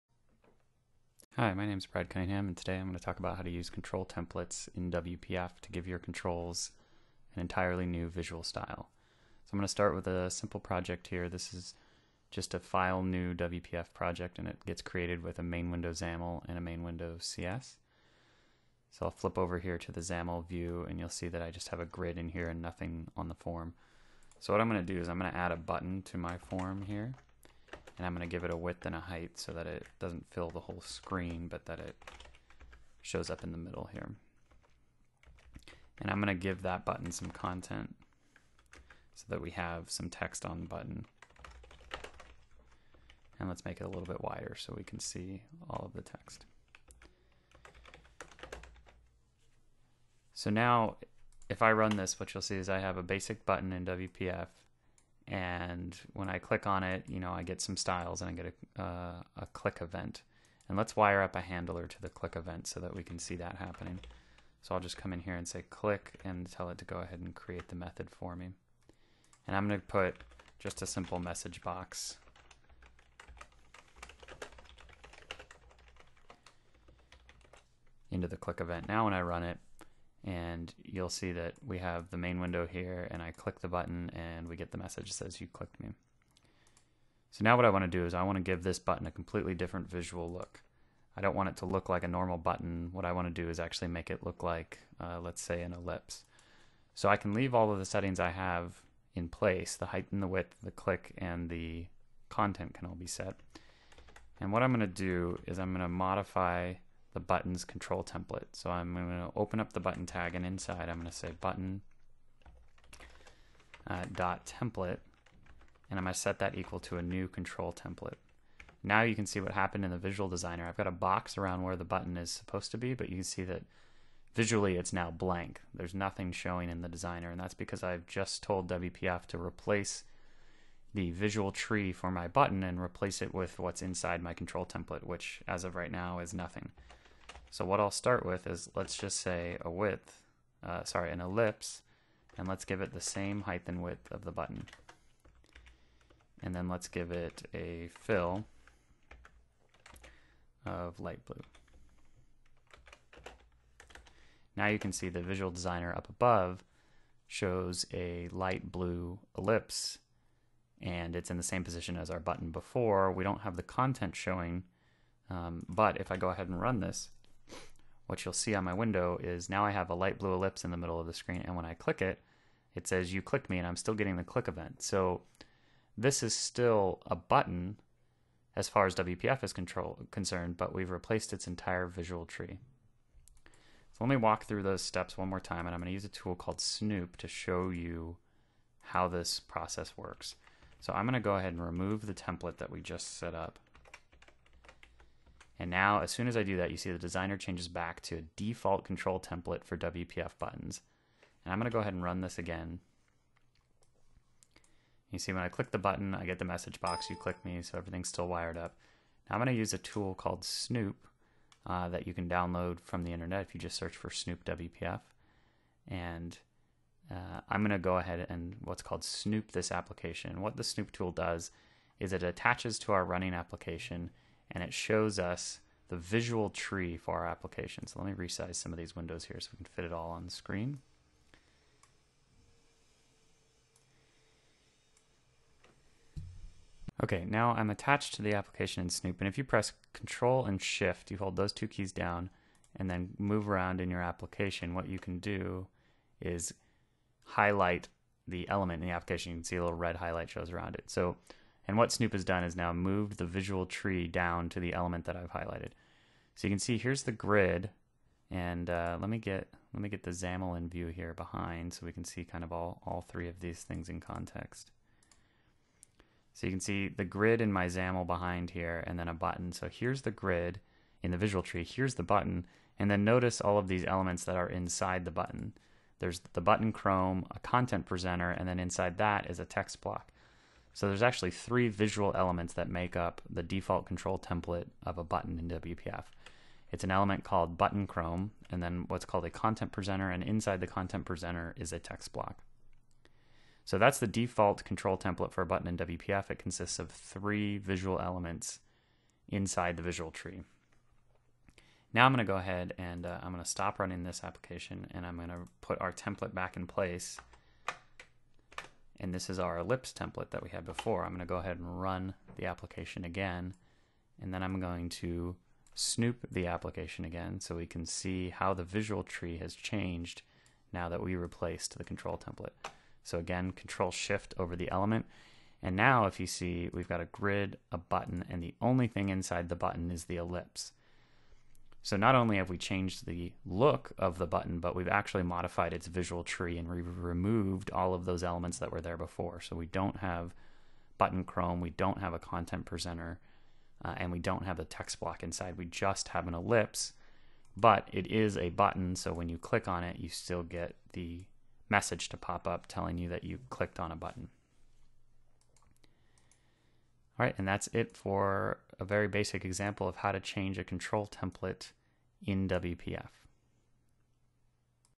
Creating ControlTemplates In WPF
This video shows you how to use a control template in WPF to change the visual tree of a button element.